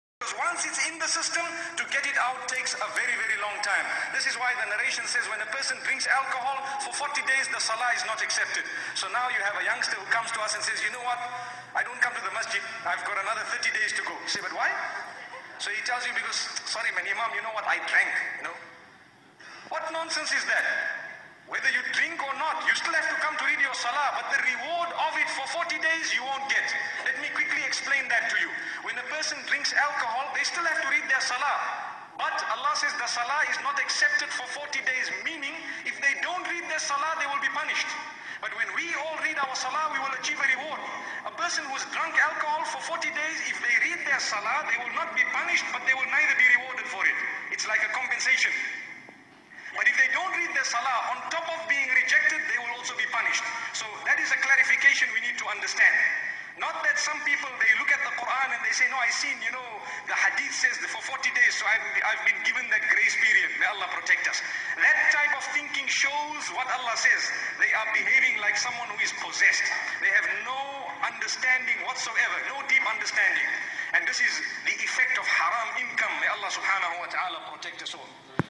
mufti menk alcohol
drank alcohol...not able to pray for 40days??